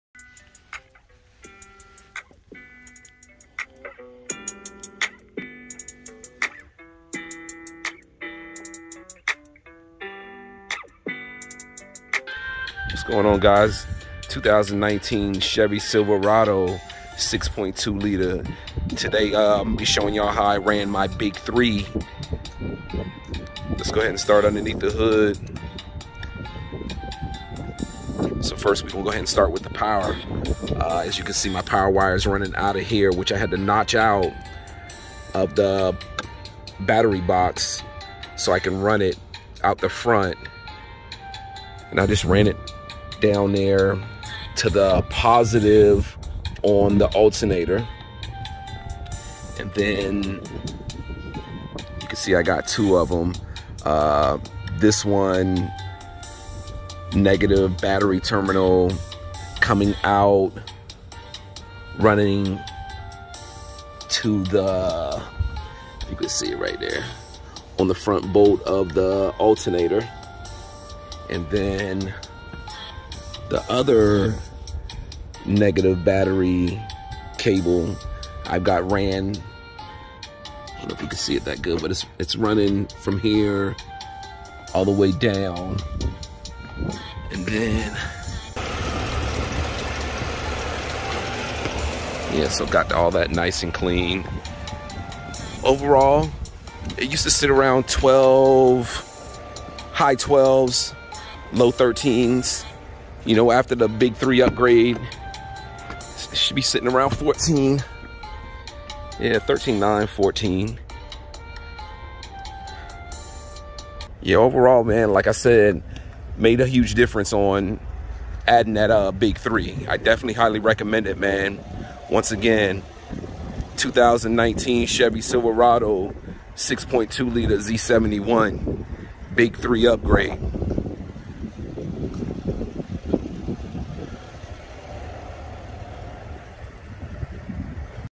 BiG 3 Upgrade 2019 CHEVY SILVERADO 6.2 l
why u should do a big 3 upgrade when having a high voltage car audio system